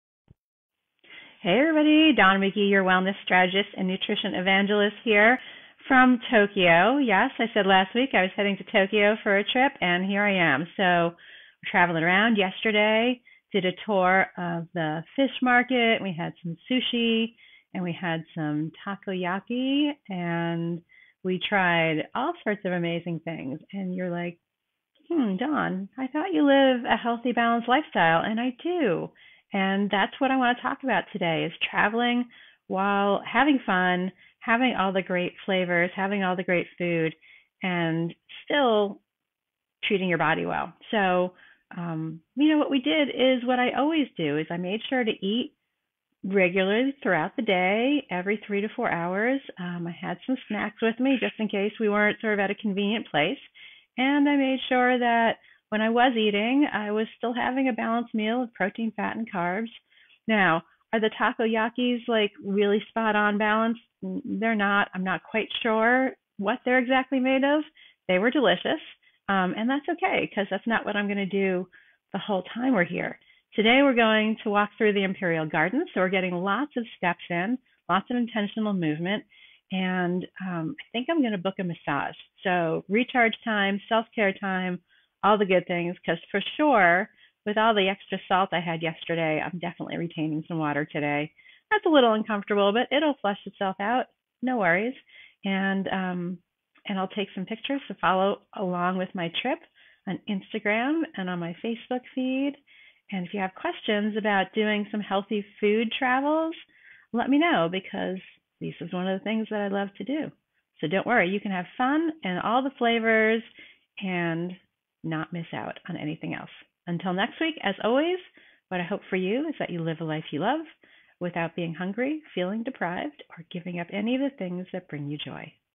TRAVEL TIPS | Balance on holiday
🌏✨ I recently wrapped up an incredible day in Tokyo, savoring all the tasty surprises! 🍣

If you're keen on finding that perfect balance between travel fun and wellness, dive into my latest blog post: "Balancing Fun and Wellness on Your Trip." 💖

Quick tips await you—think regular eats, staying active, and putting self-care first. 🌟